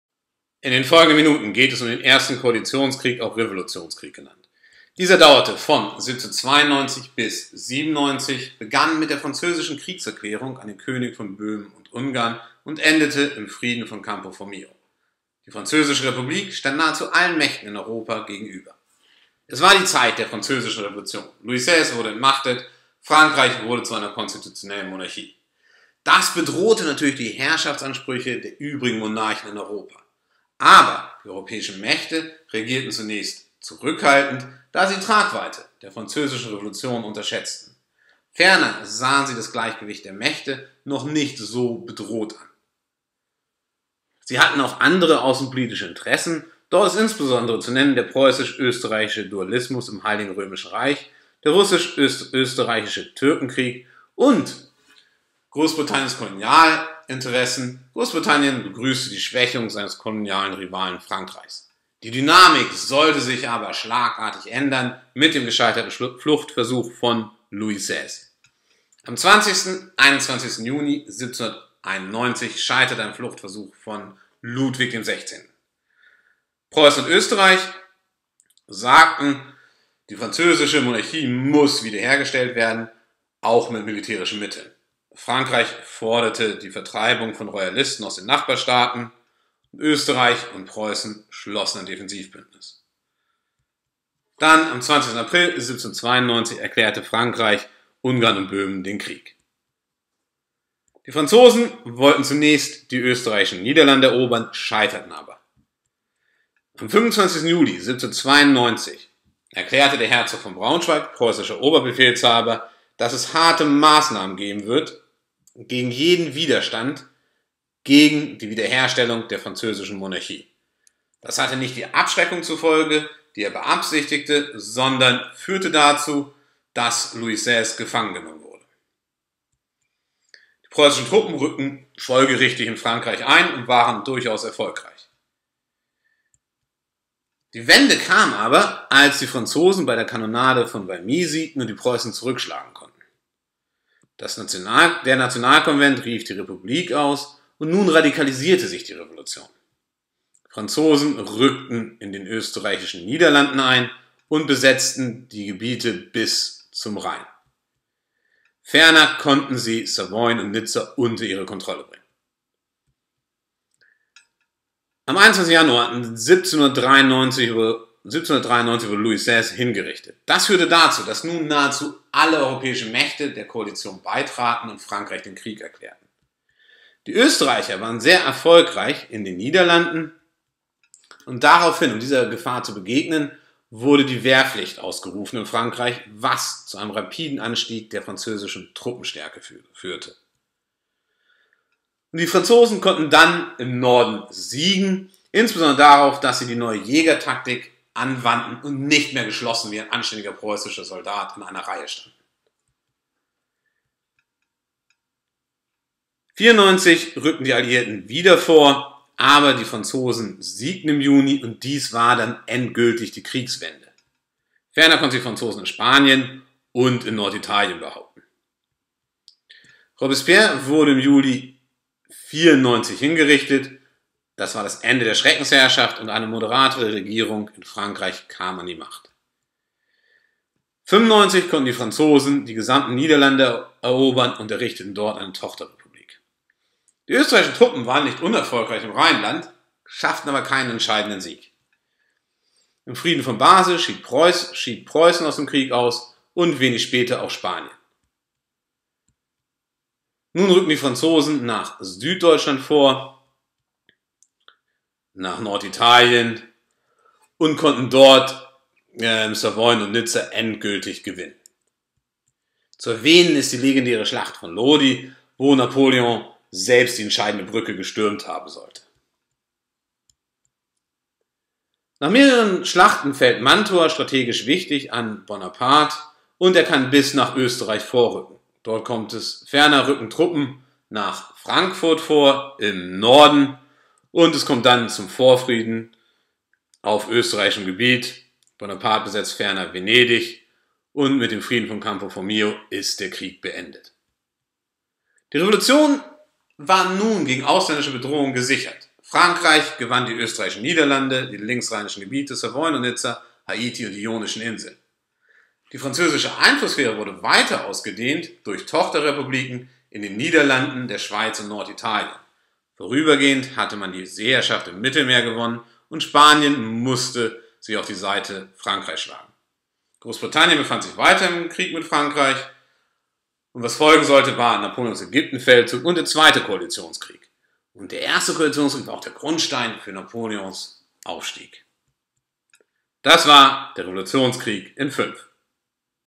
Erster Koalitionskrieg / Revolutionskrieg (1792-1797)
Im Ersten Koalitionskrieg verteidigten die Revolutionäre die Französische Revolution gegen die europäischen Mächte. Ferner konnte die junge französische Republik u.a. die Gebiete links des Rheins und Territorien in Norditalien gewinnen.